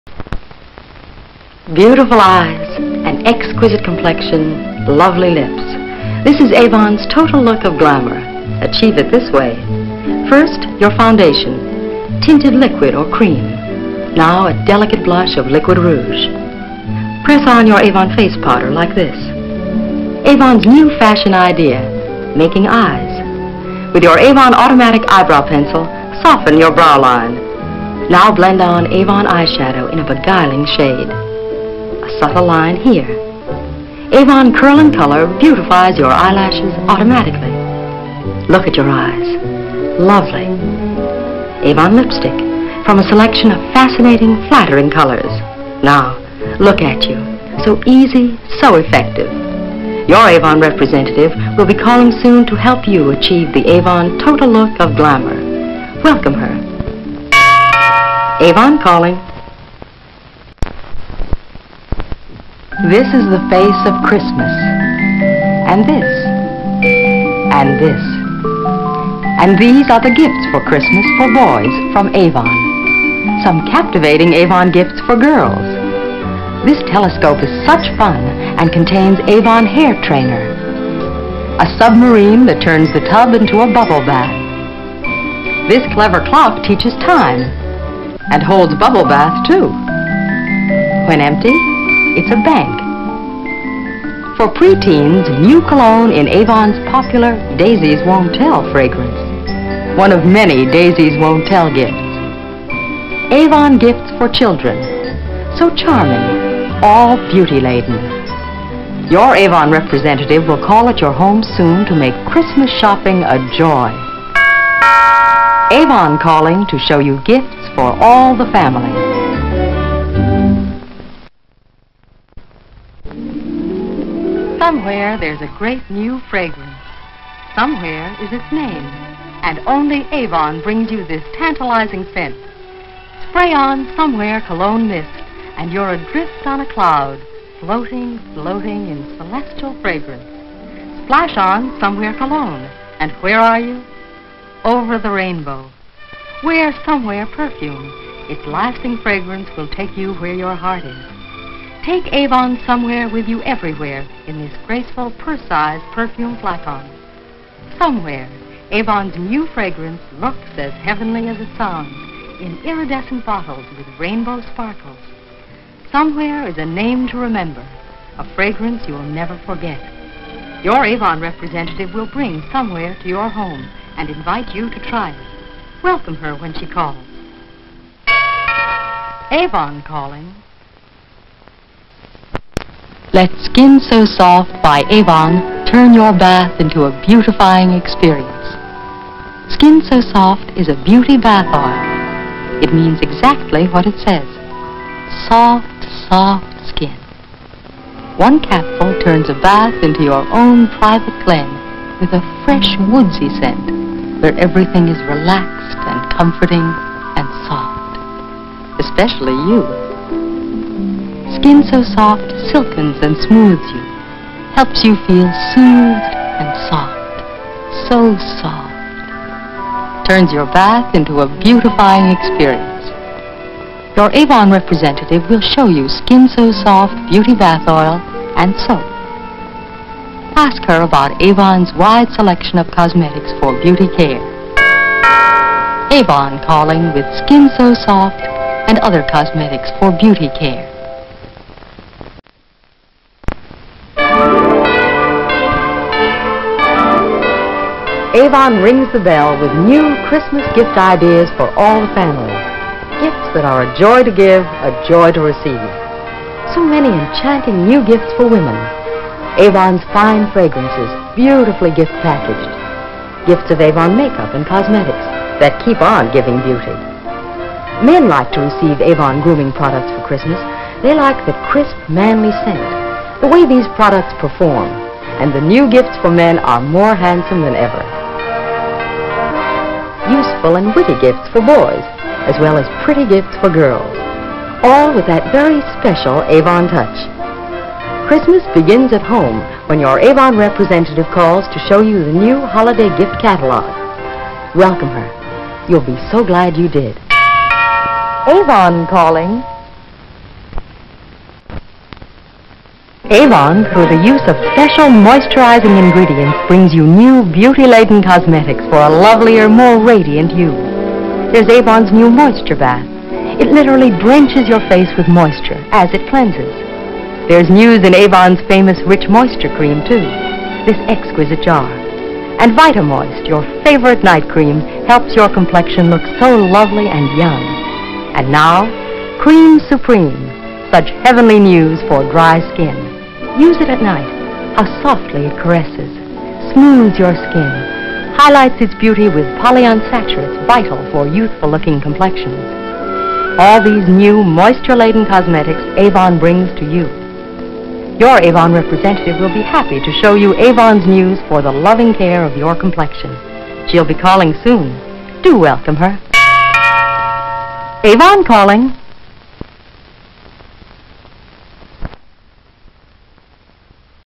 AVON Cosmetics Calling 1960's
Avon uses both door-to-door salespeople ("Avon ladies", as well as "Avon men") and brochures to advertise its products. The first Avon lady was Persis Foster Eames Albee. Avon operates training centers for potential representatives. Some Avon training centers have a small retail section with skin care products, such as creams, serums, makeup, and washes. Avon recruits sales representatives who sell beauty products, jewelry, accessories, and clothing.